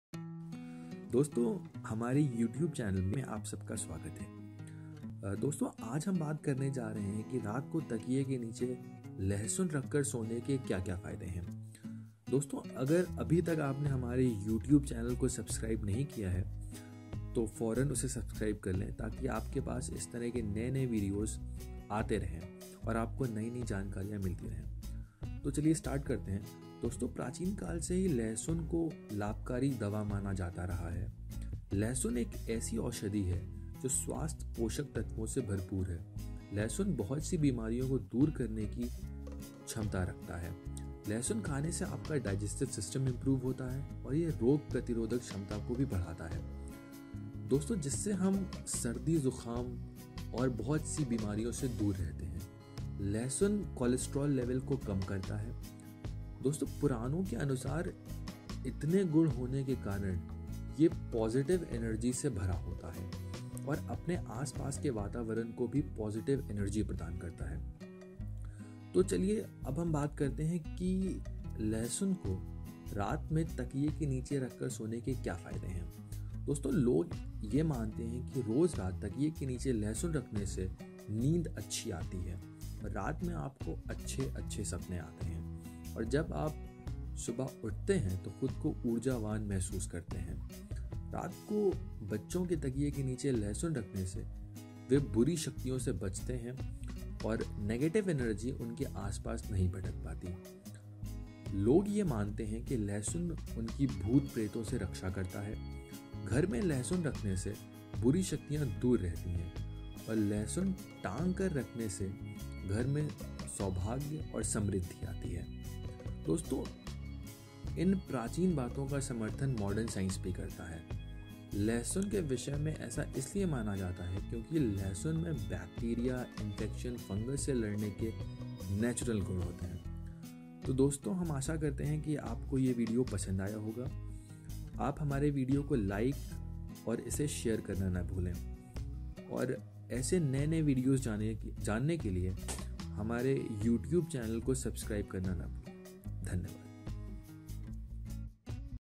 क्या आप जानते हैं ? रात को तकिये के नीचे लहसुन रखने से होंगे चमत्कारी फायदे - Benefits of Garlic
क्या आप जानते हैं ? रात को तकिये के नीचे लहसुन रखने से होंगे चमत्कारी फायदे - Benefits of Garlic clove under your pillow